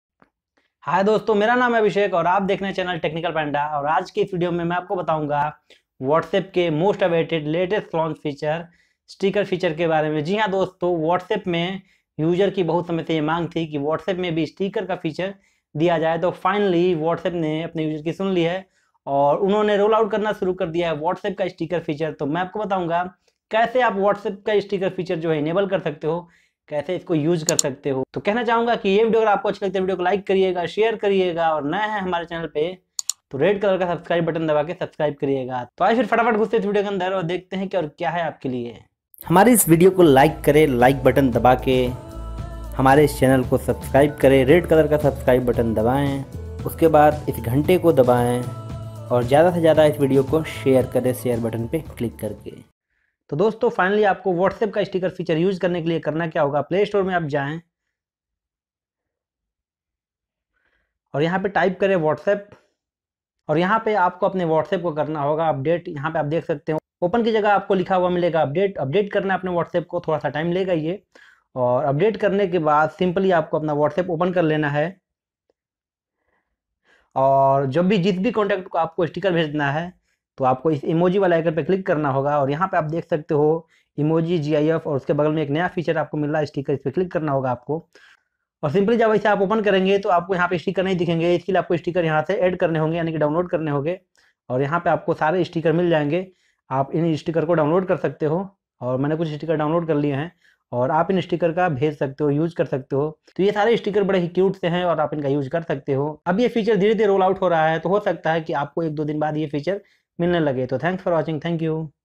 how to send sticker in whatsapp|whatsapp me sticker kaise bheje
TO DOSTO=

To dosto final whatsapp ne apne letest feature whatsapp stickers feature jo hai rolout lounch kar diya hai.
to mai batauga ke kaise aap whatsapp ka ye wala feature stickers kaise paa sakte ho
 thanks

This Video Covers Below Topics-
1.How To Eanble whatsapp Sticker Feature.
2.Whatsapp Sticker Not Showing.
3.Download Whatsapp Sticker
4. Whatsapp me sticker kaise paye

-Agar apko ye video achi lagi to like zaroor karna video ko
-Video ko apne friends or family ke saath share karna